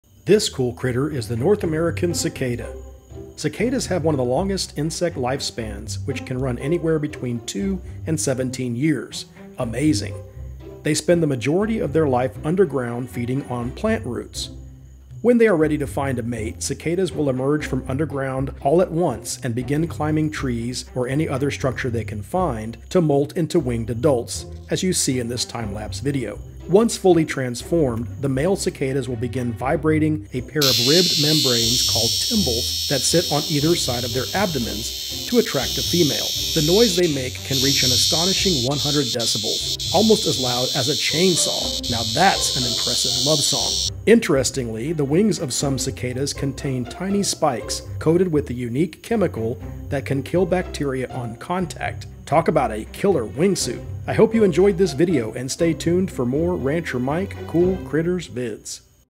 COOL CRITTERS! - What's With All That NOISE?!? - Cicada
Why do cicadas emit so much noise?!? In this video, I'll tell you why cicadas are so noisy and reveal some other interesting facts about this truly unique insect!

Like something you see in one of my videos? You can shop my recommended Amazon products